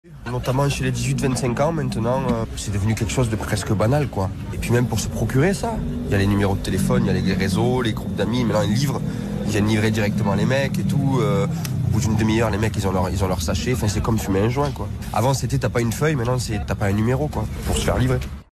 La cocaïne, une drogue banalisée ?
🔴🎙La cocaïne, une drogue banalisée ? Hugo Amelin, le correspondant de RTL à Marseille, a recueilli le témoignage de Maxime, un noctambule phocéen, sur les usages de consommation de la cocaïne aujourd’hui. Rendez-vous dès maintenant sur rtl.fr et l’application RTL pour écouter le reportage📱💻